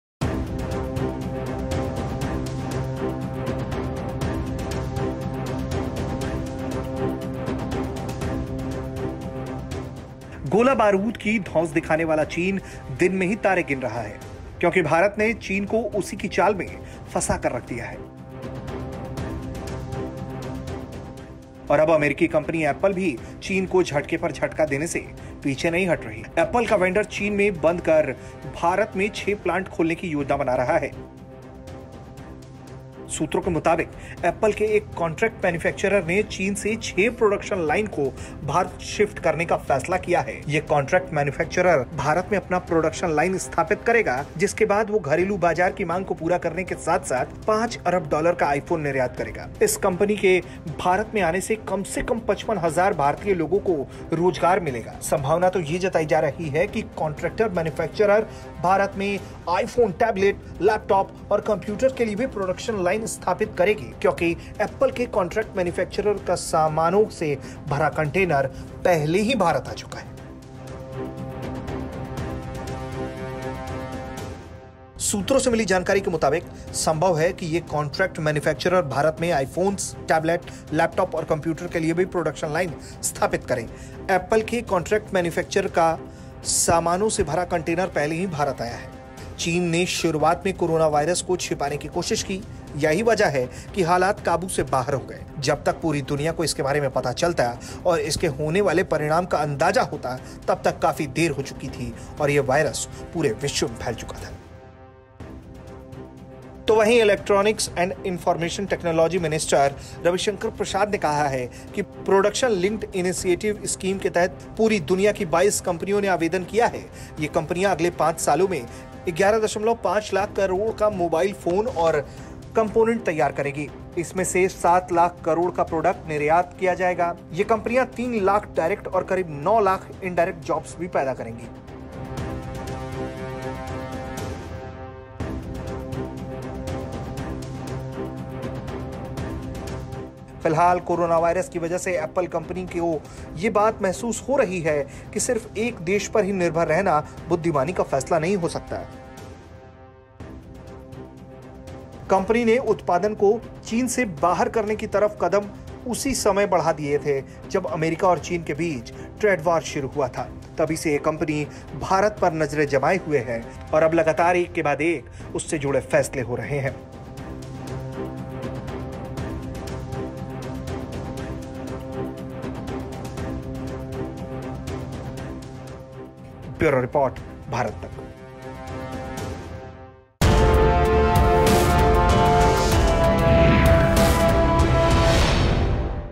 Apple का वेंडर चीन में बंद कर भारत में खोलेगा 6 प्लांट, 55000 लोगों को मिलेगा रोजगार
गोला-बारूद की धौंस दिखाने वाला चीन दिन में ही तारे गिन रहा है क्योंकि भारत ने चीन को उसी की चाल में तोड़कर रख दिया और अब अमेरिकी कंपनी एप्पल भी चीन को झटके पर झटका देने से पीछे नहीं हट रहा है। एप्पल का वेंडर चीन में बंद कर भारत में 6 प्लांट खोलने की योजना बना रहा है। सूत्रों के मुताबिक एप्पल के एक कॉन्ट्रैक्ट मैन्युफैक्चरर ने चीन से 6 प्रॉडक्शन लाइन को भारत शिफ्ट करने का फैसला किया है और ये कॉन्ट्रैक्ट मैन्युफैक्चरर भारत में अपना प्रॉडक्शन लाइन स्थापित करेगा जिसके बाद वो घरेलू बाजार की मांग को पूरा करने के साथ-साथ 5 अरब डॉलर का आईफोन निर्यात भी करेगा। इस कंपनी के भारत में आने से कम से कम 55 हजार भारतीय लोगों को रोजगार मिलने की संभावना है।